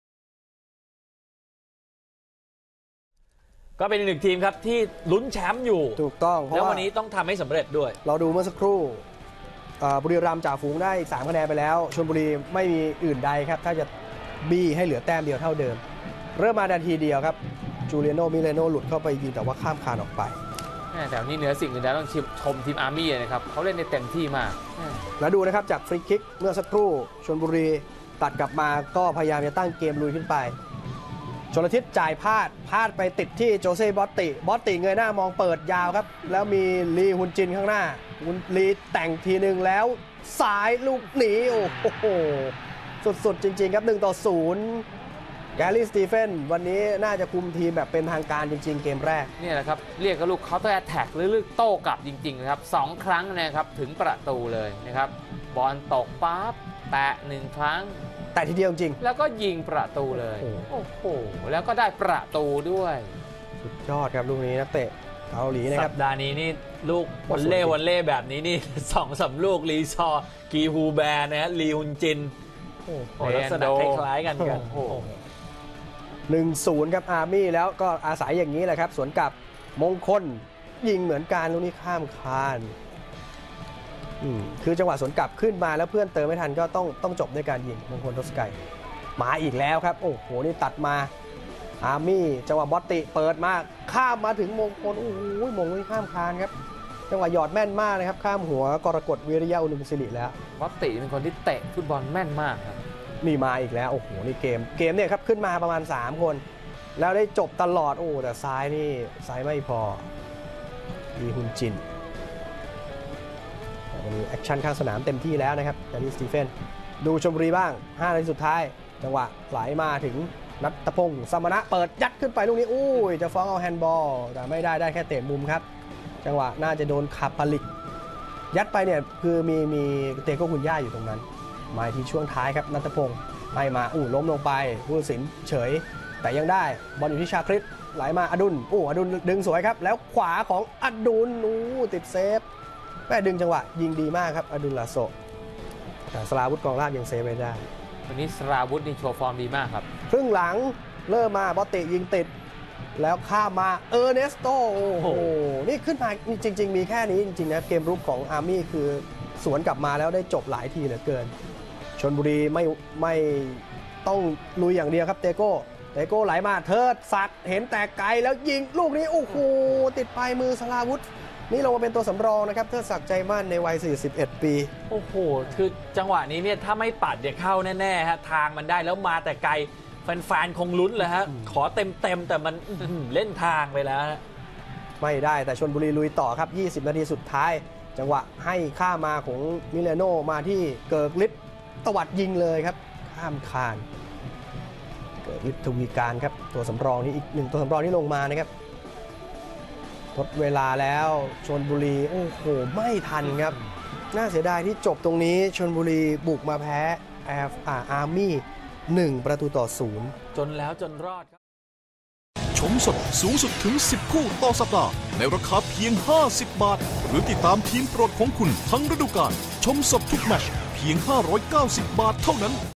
TPL 2014 - Army United 1 - 0 Chonburi (15-10-2014)
Army United 1 - 0 Chonburi
นาทีที่ 9  Hyunjin Lee     Army United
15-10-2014